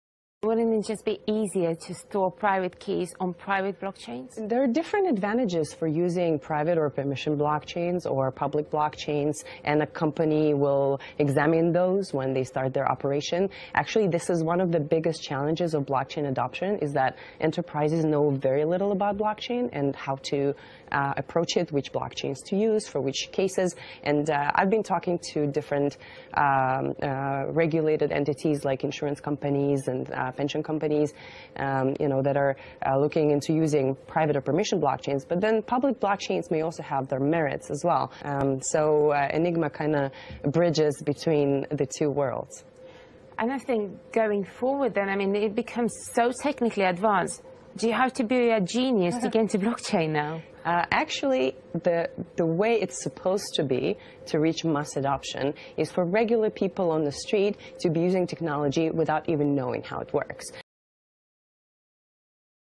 Can enterprises really trust public blockchains?
Wouldn't it be easier to store private keys on private blockchains? It’s a valid question—but the real issue lies deeper. Most enterprises don’t yet understand which type of blockchain suits their needs. Public vs. private chains each offer unique advantages, and platforms like Enigma aim to bridge the two. The goal? A future where blockchain works quietly in the background—so seamlessly that users don’t even realize it's there. Because true mass adoption won’t require people to be tech geniuses. It will require blockchain to feel effortless.